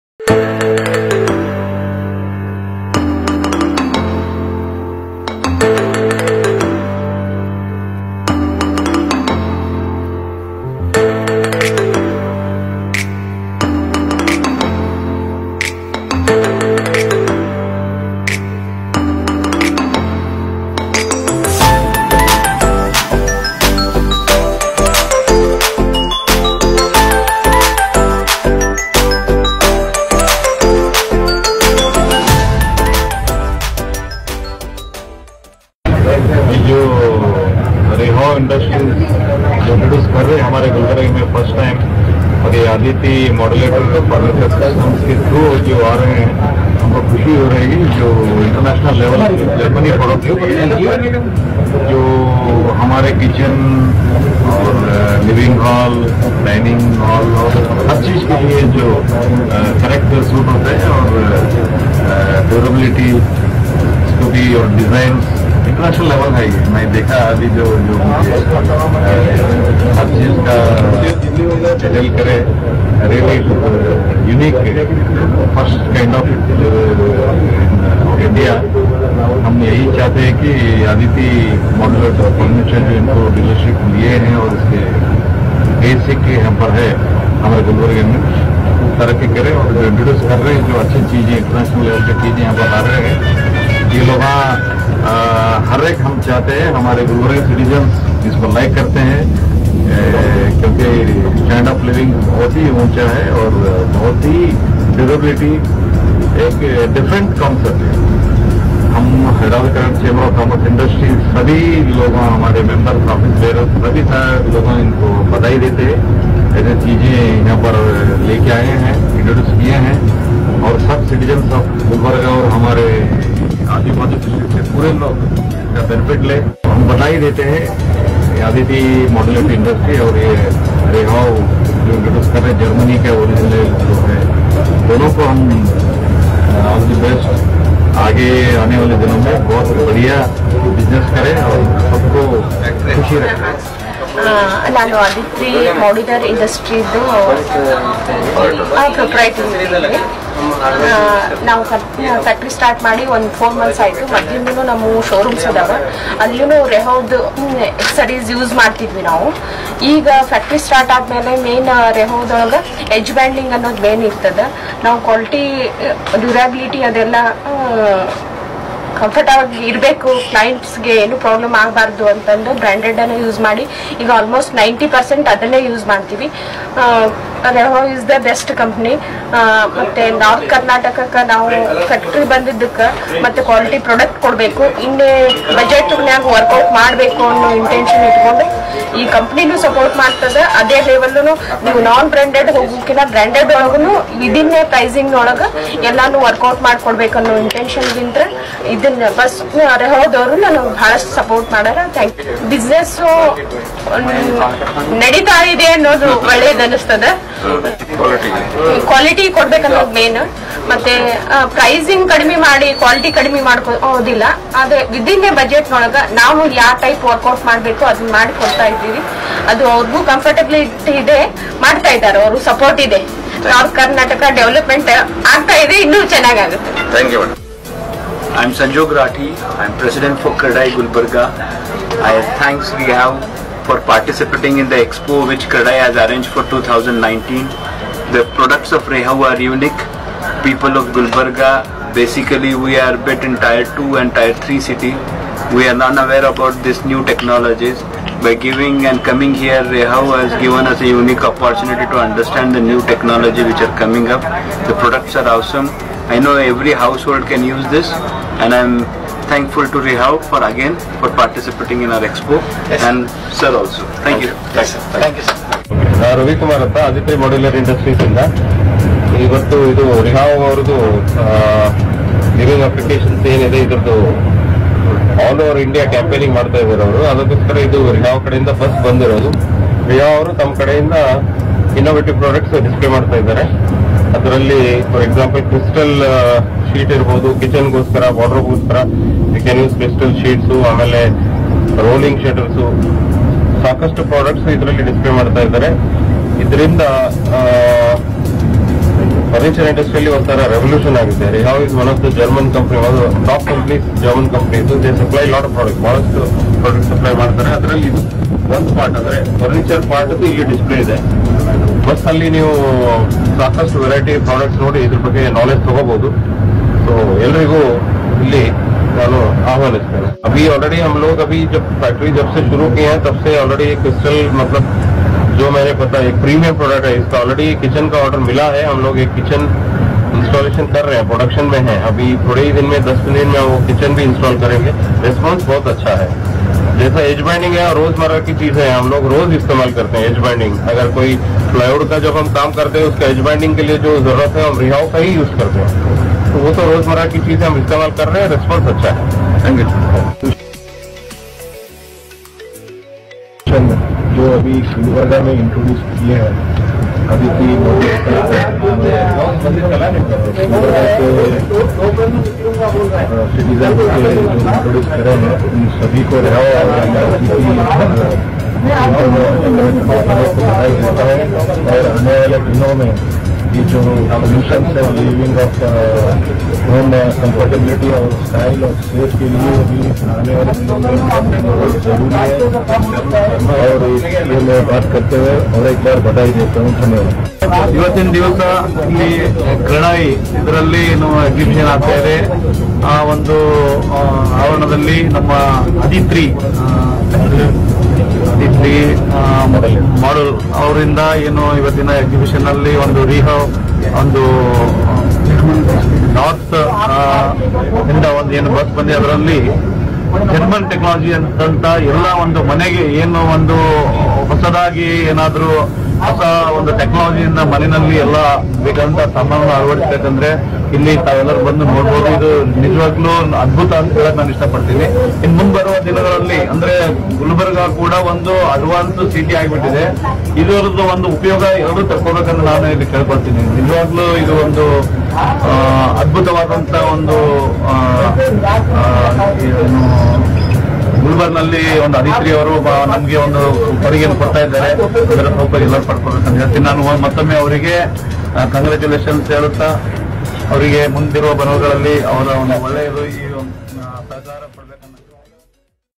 ADITRI Modular Industries
ADITRI MODULAR INDUSTRIES ASIAN BUSINESS CENTRE

HAS BROUGHT BEST BRAND REHAU FOR GOOD LIFE PERFECT FINISHING OF FURNITURE

ADITRI MODULAR INDUSTRY TOOK PART IN CREDAI EXPO AND BRAND REHAU EXPRESS IN GULBARGA

ON THIS OCCASION HKCC PRESIDENT AMARNATH PATIL,CREDAI PRESIDENT SANJOG RATHI & OTHER DIGNITARIES PRESENT

ADRESS ASIAN BUSINESS CENTRE
GULBARGA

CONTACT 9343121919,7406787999